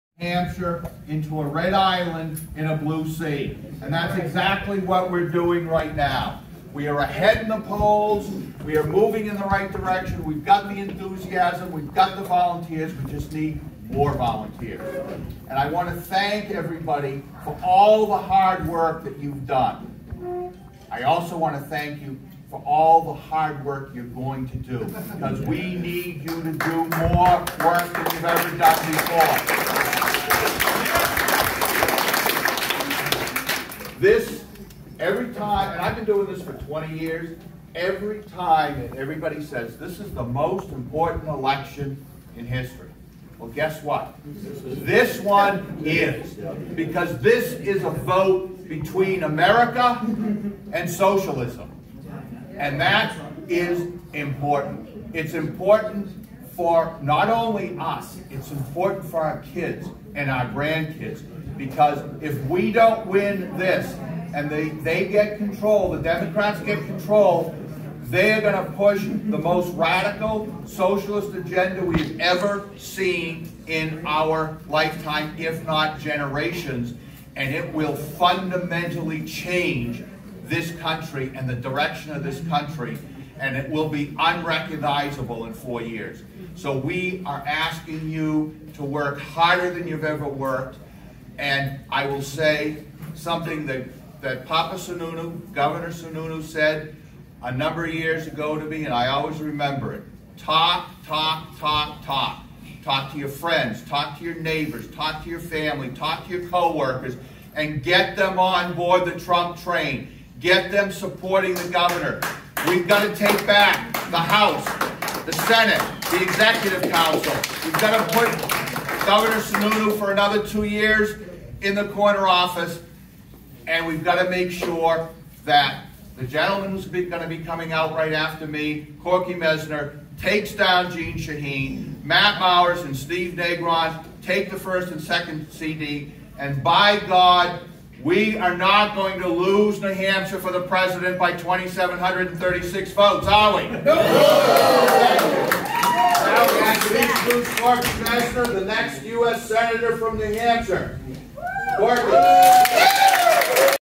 2020-09-29 debate watch party trump HQ Manchester NH 1 Chair of NH Republican State Committee
Friend,

The Trump Victory Team will be hosting a Debate Night Watch Party TONIGHT September 29th!

We will be watching the FIRST Presidential Debate as we rally together to flip New Hampshire RED. Trump t-shirts, bumper stickers, and other swag will be available on a first-come-first-served basis.

Join us with Special Guests, Matt Mowers for Congress, Corky Messner for Senate, and Donald J. Trump for President, Inc. Director of Press Communications Erin Perrine!

For further information and to RSVP, sign up below to attend TONIGHT, at 7:30 PM, at the Manchester Trump Victory office, 377 S. Willow St. Unit B1-7 Manchester, NH 03103.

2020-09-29 debate watch party trump HQ Manchester NH 5 Director of Press Communications Erin Perrine